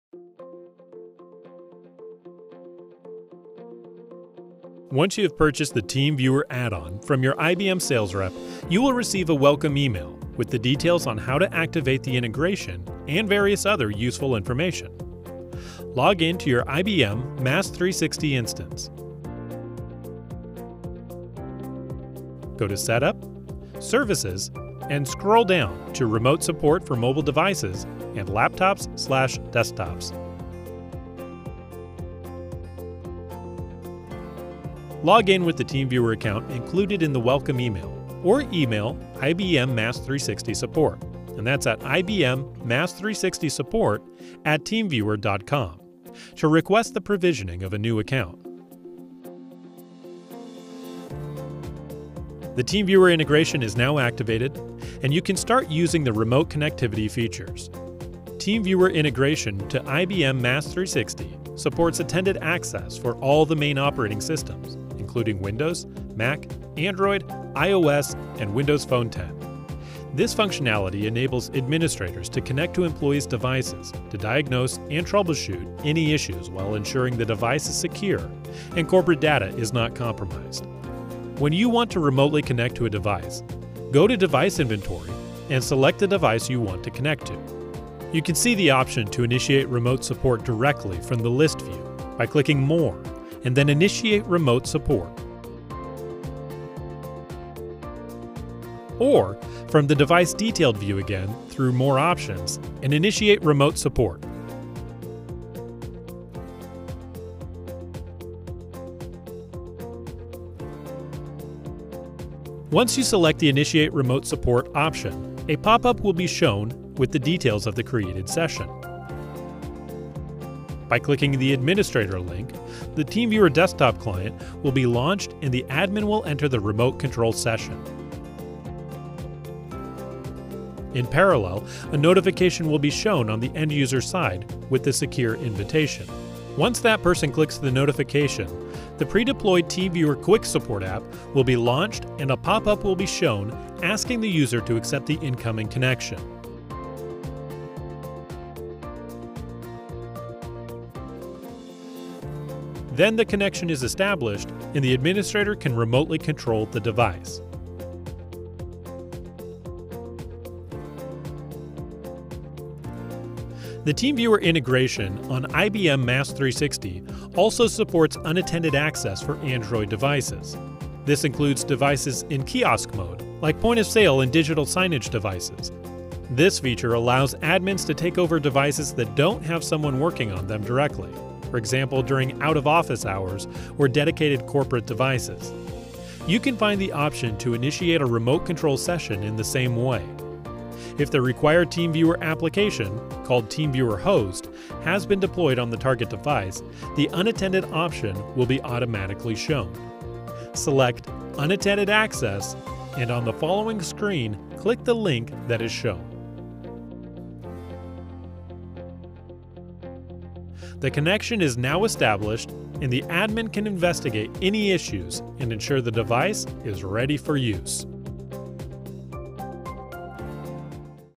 TeamViewer integration to IBM MaaS360
HowTo video describing how to enable and use the TeamViewer remote control integration to IBM MaaS360 platform, supporting both attended and unattended access.